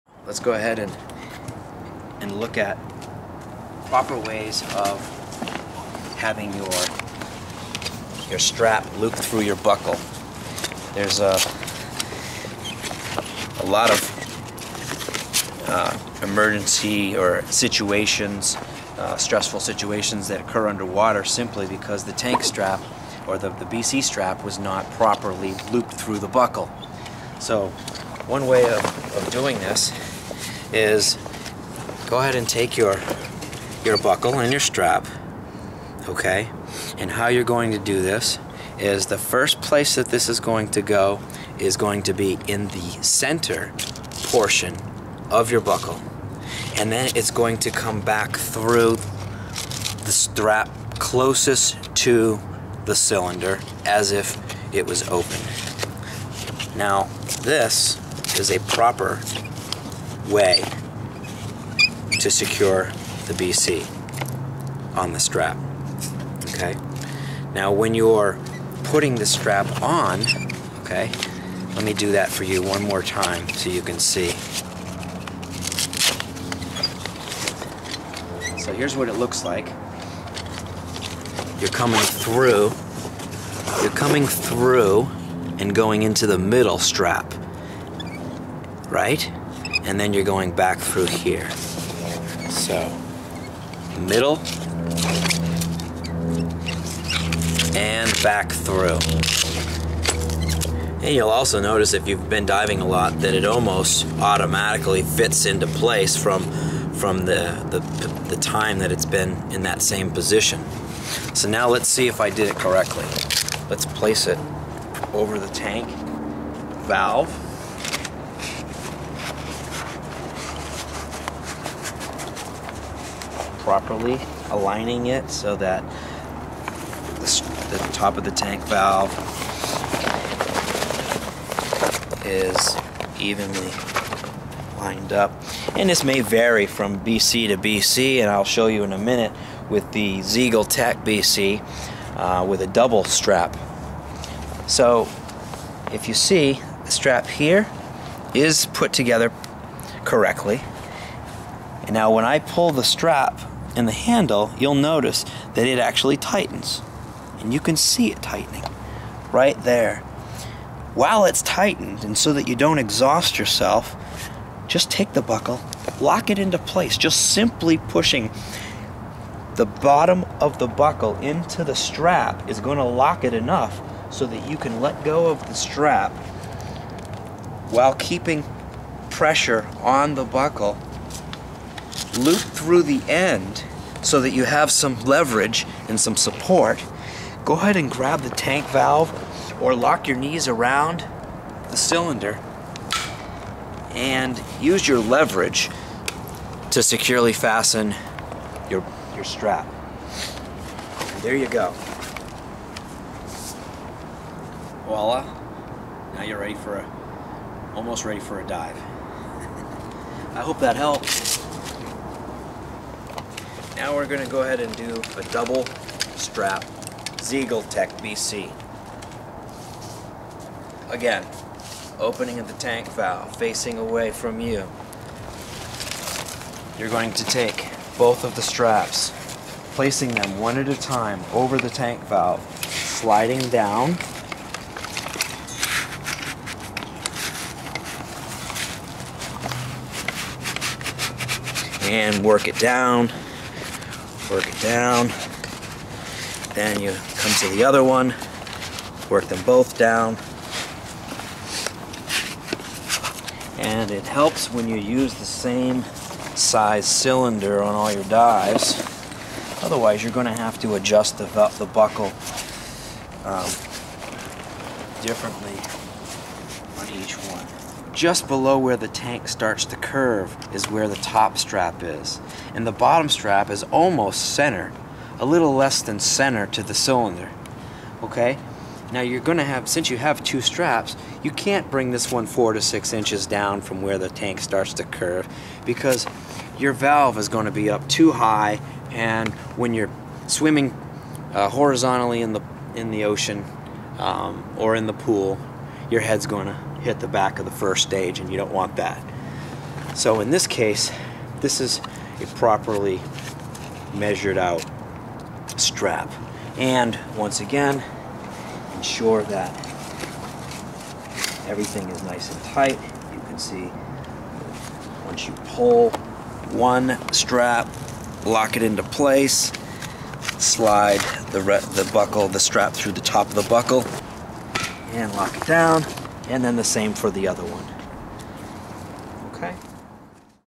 SCUBA BCD Tank Strap & Buckle Safe Assembly.mov
SCUBA BCD Tank Strap & Buckle Assembly Demonstration

Avoid difficult or potentially dangerous situations underwater by following this one simple procedure.

 I can certify you in Open Water Diver to Assistant Instructor. Learn to dive or take your diving to the next level.

Follow along as we assemble a basic SCUBA set-up.